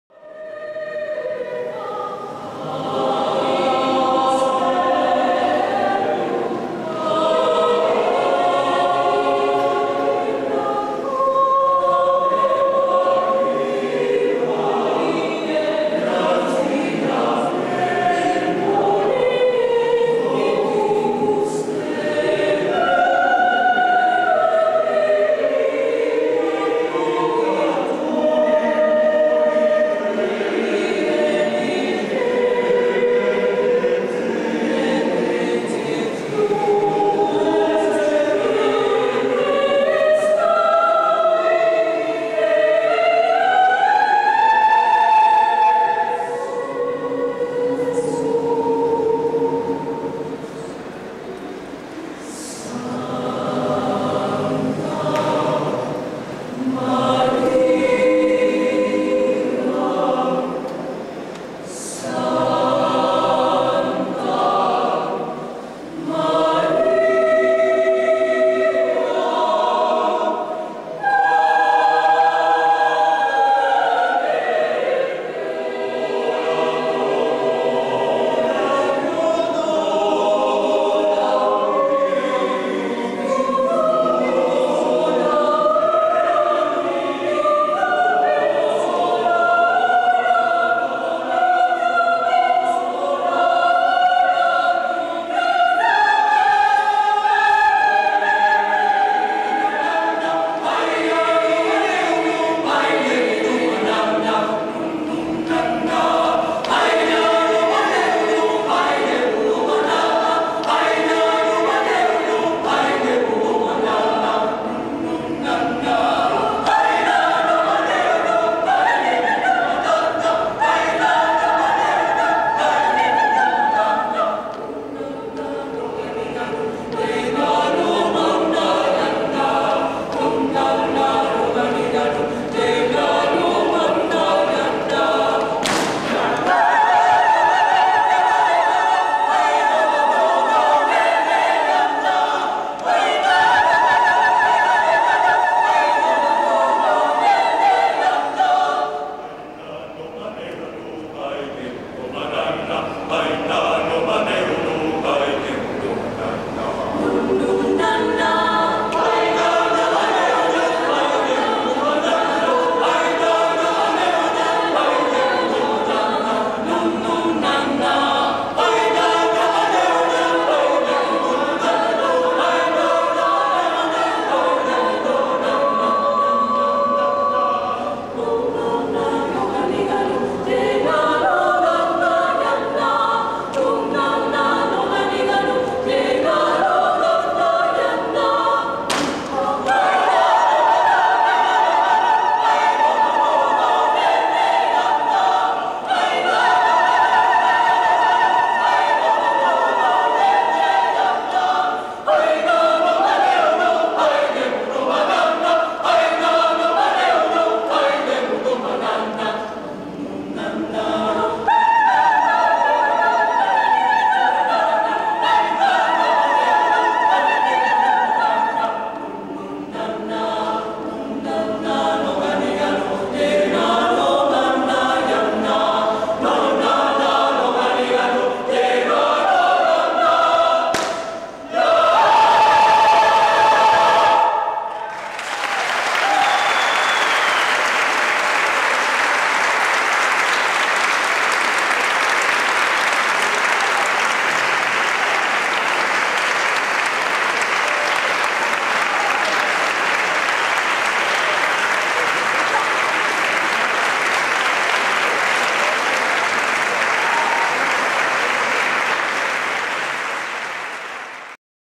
CANTEMUS IX FESTIVAL DE COROS BARRANQUILLA 2009
CANTEMUS Director Carlos Bastos EN EL IX FESTIVAL DE COROS BARRANQUILLA 2009.
Iglesia tres ave maria